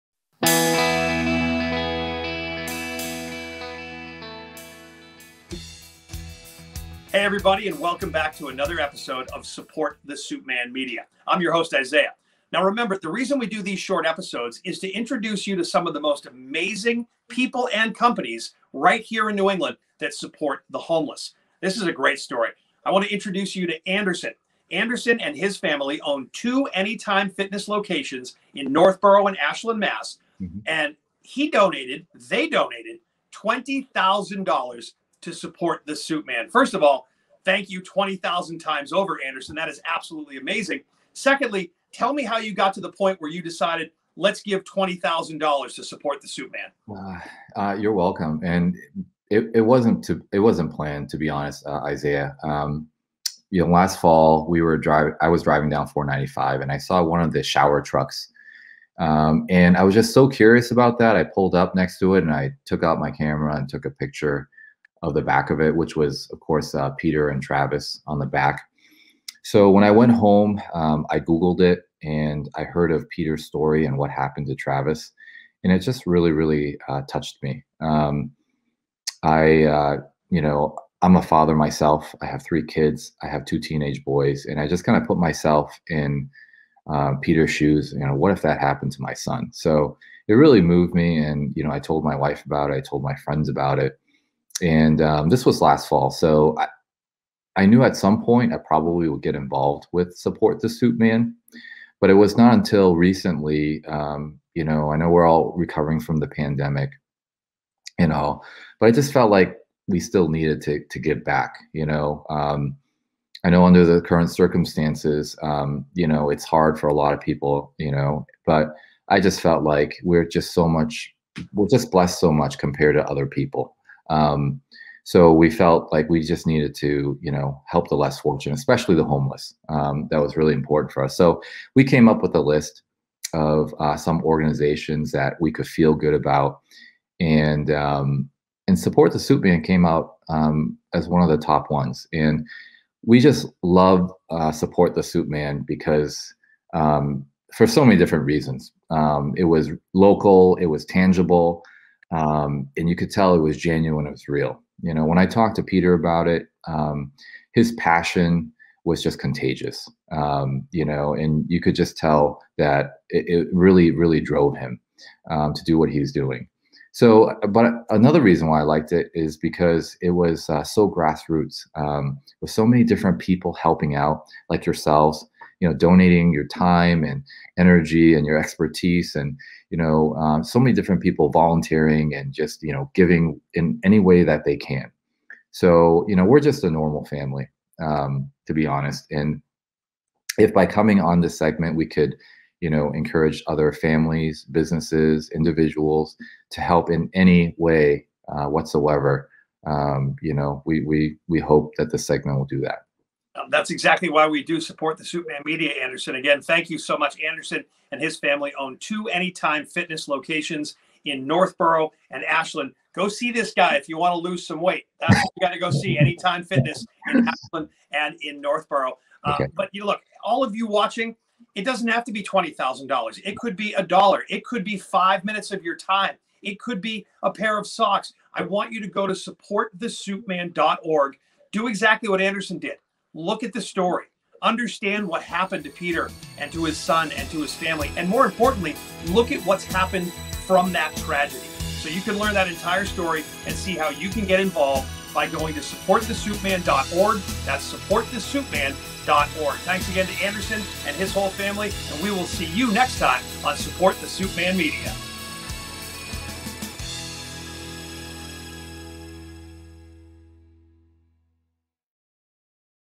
Soupman Media Series - Episode 6 | Anytime Fitness
Another Support The Soupman soupmanmedia installment, hosted by ⭐️Isaiah Moskowitz⭐️ Listen in as Anderson Huang owner of Anytime Fitness in Northboro and Ashland MA describes the unusual manner in which he and his wife opted to donate 20k to help New England's homeless!